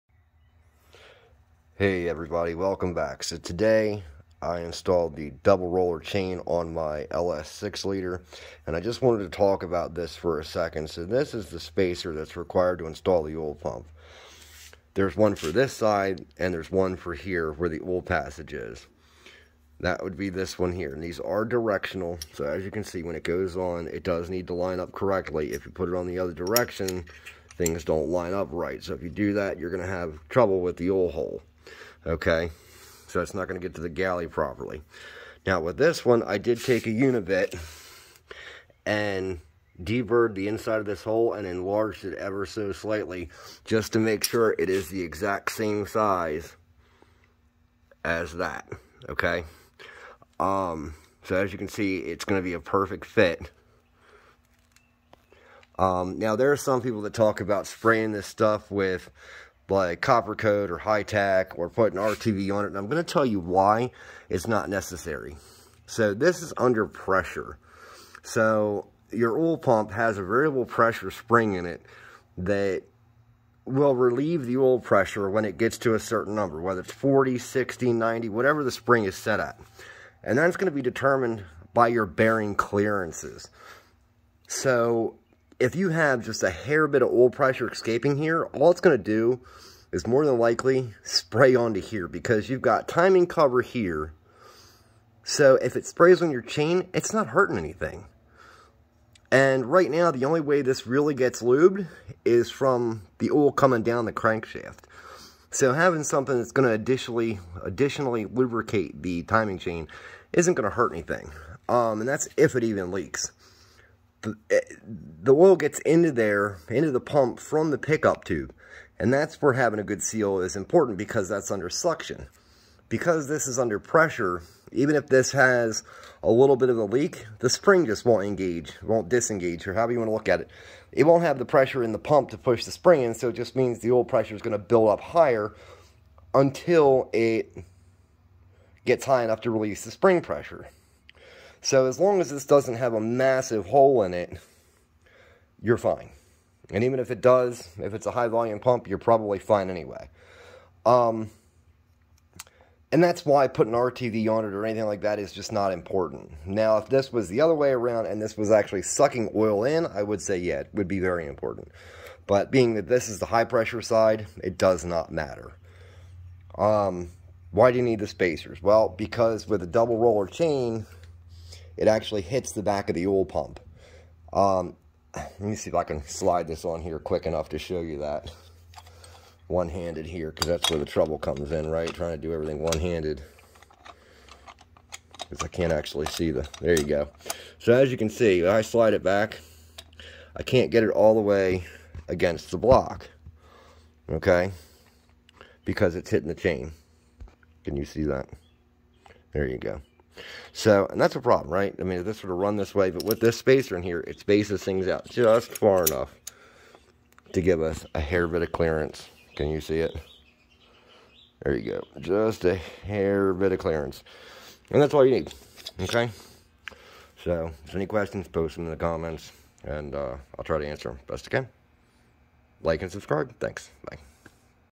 LS Oil Pump Spacer Install. Do you need RTV on it?
in this really short video I will explain why it is not important to have our TV or high tech or copper gasket coat on the oil pump spacer that used when installing a double roller timing chain on an LS engine.